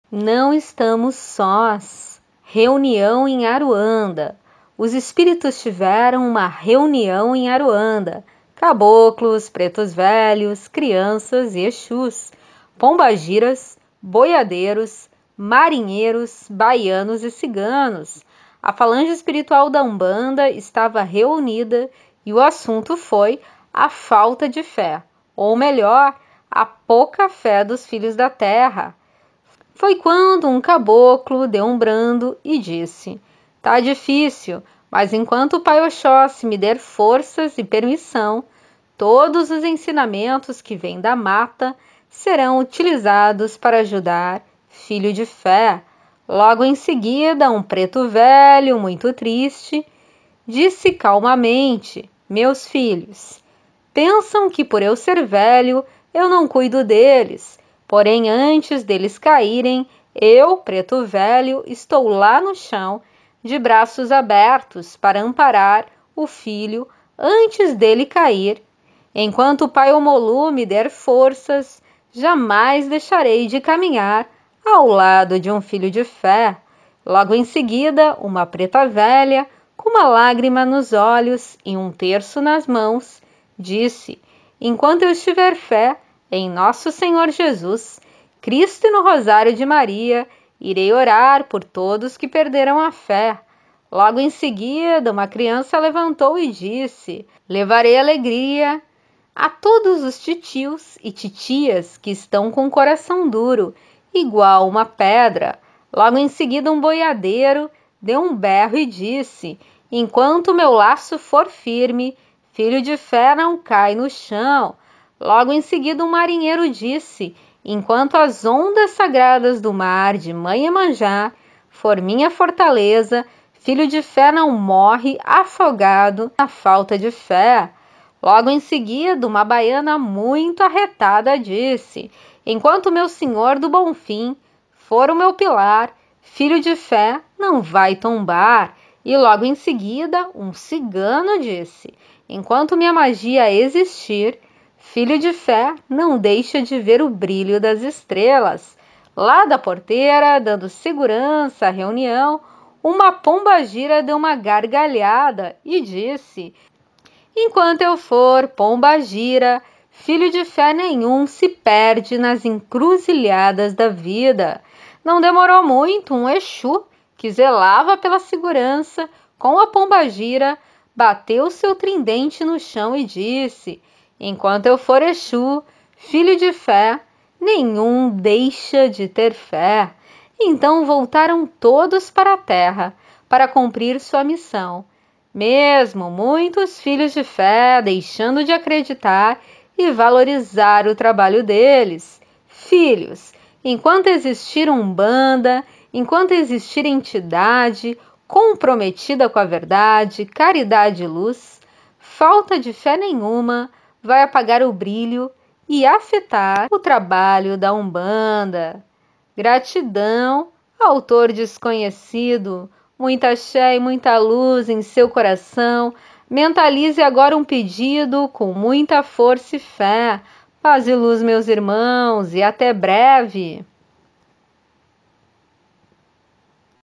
A reunião dos orixás em Aruanda ( Não estamos só)
Texto muito bonito sobre o amor dos nossos orixás.